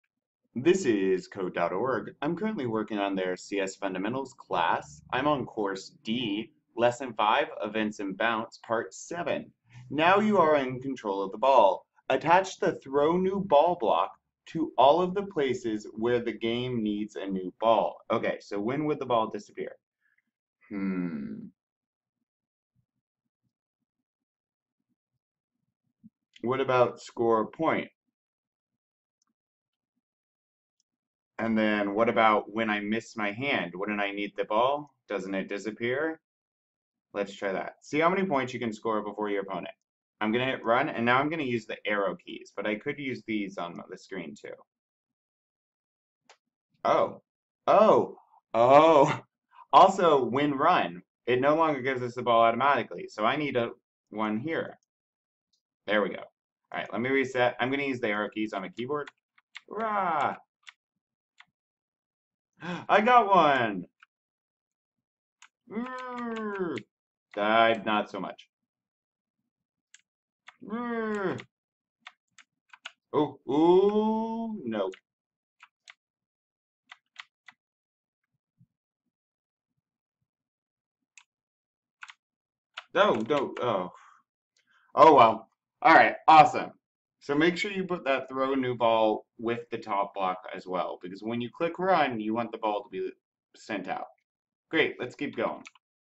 Code.org Lesson 6.7 Events in Bounce | Answers Explained | Course D
Events in Bounce, lesson 6, part of C.S. Fundamentals Course D from Code.org. It is an excellent way to learn coding. Each part challenges students to accomplish a new task while learning computer science concepts. This particular course, Course D is often used in third grade classrooms. In this video I walk through step-by-step how to get the right answer for this puzzle. Tutorials for more lessons are linked below.